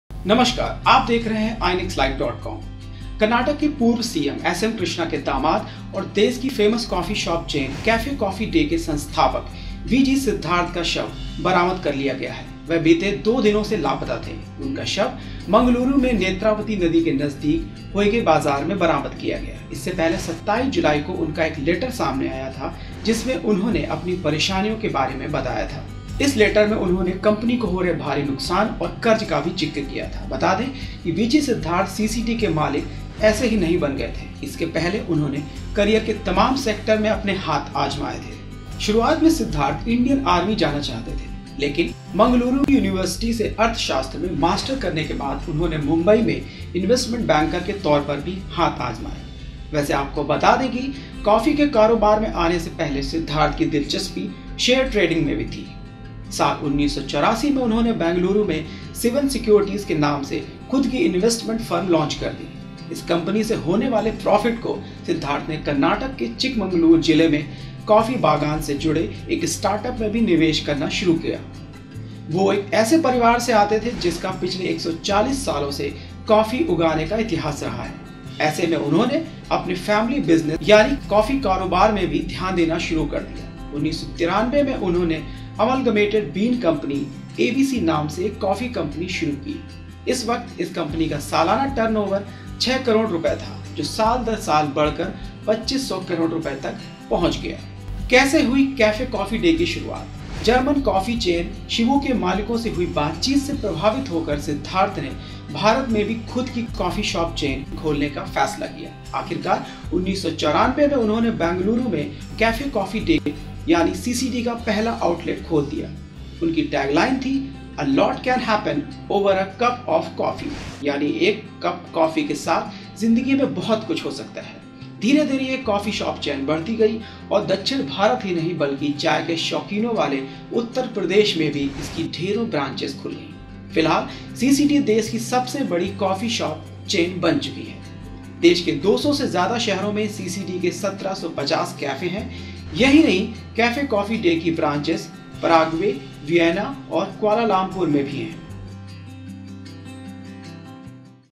VG Siddhartha: CCD के मालिक बनने का सफर
कर्नाटक के पूर्व सीएम एसएम कृष्णा के दामाद और देश की फेमस कॉफी शॉप चेन कैफे कॉफी डे  के संस्थापक वीजी सिद्धार्थ का शव बरामद कर लिया गया है। वह बीते दो दिनों से लापता थे। उनका शव मंगलुरू में नेत्रावती नदी  के नजदीक होइगे बाजार में बरामद किया गया। इससे पहले 27 जुलाई को उनका एक लेटर सामने आया था, जिसमें उन्‍होंने अपनी परेशानियों के बारे में बताया था। इस लेटर में उन्होंने कंपनी को हो रहे भारी नुकसान और कर्ज का भी जिक्र किया था। बता दें कि वीजी सिद्धार्थ सीसीडी के मालिक ऐसे ही नहीं बन गए थे, इसके पहले उन्‍होंने करियर के तमाम सेक्‍टर में अपने हाथ आजमाए थे। जानिए उनका ये सफर.